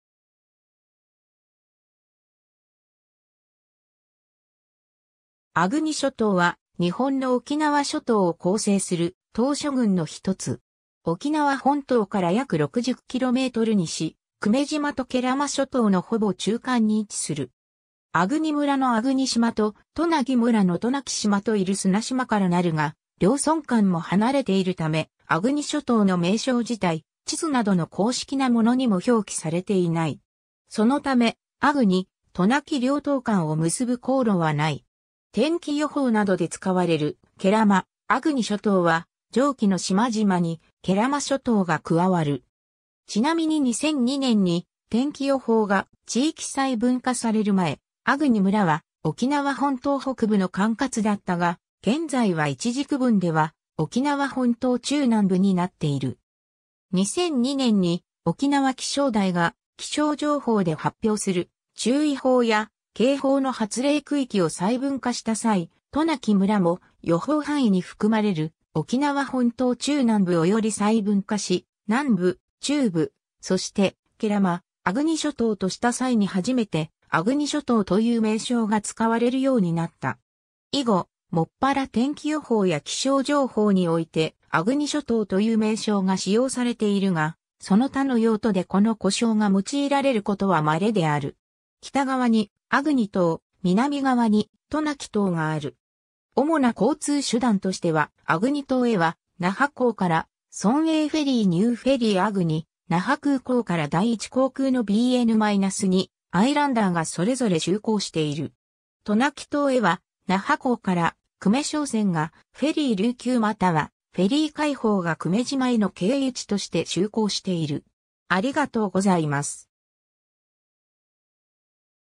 粟国諸島
粟国諸島（あぐにしょとう）は、日本の沖縄諸島を構成する島嶼群の一つ。
沖縄本島から約60km西、久米島と慶良間諸島のほぼ中間に位置する。
 粟国村の粟国島と、渡名喜村の渡名喜島と入砂島（無人島）から成るが両村間も離れているため「粟国諸島」の名称自体、地図などの公式なものにも表記されていない。
そのため粟国・渡名喜両島間を結ぶ航路はない。
 天気予報などで使われる「慶良間・粟国諸島」は上記の島々に慶良間諸島が加わる。
ちなみに2002年に天気予報が地域細分化される前、粟国村は「沖縄本島北部」の管轄だったが、現在は一次区分では「沖縄本島中南部」になっている。
 2002年に沖縄気象台が気象情報で発表する注意報や警報の発令区域を細分化した際、渡名喜村も予報範囲に含まれる「沖縄本島中南部」をより細分化し「（沖縄本島）南部」（浦添市・西原町以南の本島南部）、「（沖縄本島）中部」（宜野湾市・中城村以北の本島中部）、そして「慶良間・粟国諸島」（渡嘉敷村・座間味村・粟国村・渡名喜村）とした際に初めて「粟国諸島」という名称が使われるようになった。
 以後、もっぱら天気予報や気象情報において「粟国諸島」という名称が使用されているが、その他の用途でこの呼称が用いられることは稀である。
 北側に粟国島、南側に渡名喜島がある。
主な交通手段としては粟国島へは那覇港（泊ふ頭）から村営フェリー・ニューフェリー粟国、那覇空港から第一航空のBN-2アイランダー（9人乗り）がそれぞれ就航している。
渡名喜島へは那覇港（泊ふ頭）から久米商船がフェリー琉球又はフェリー海邦が久米島（兼城港）への経由地として就航している。